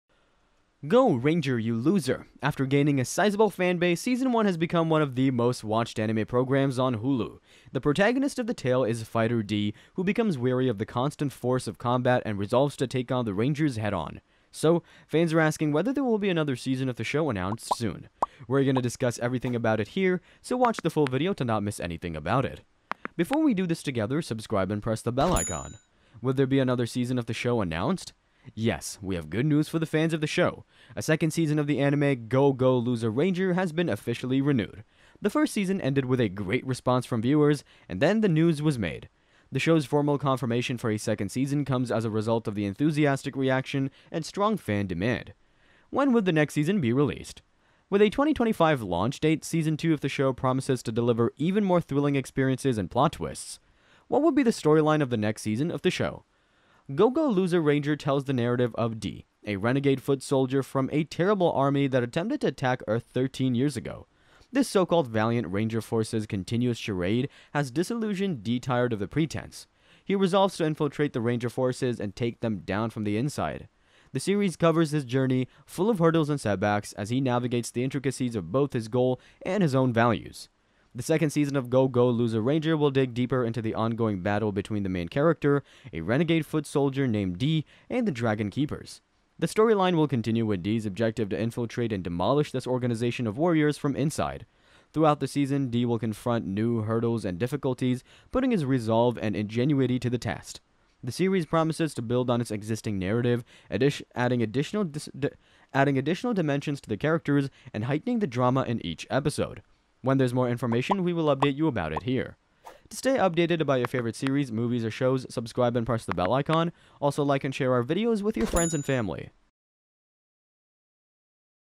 Go! Go! Loser Ranger! Season 2 Trailer | Release Date | All The Major Updates!!
Go! Go! Loser Ranger! Season 2 -

as for "go go loser ranger" season 2, there’s no official news yet, but it looks promising! given the success of other anime adaptations from shonen magazine titles, there's a good chance the show will be renewed. stay tuned for updates!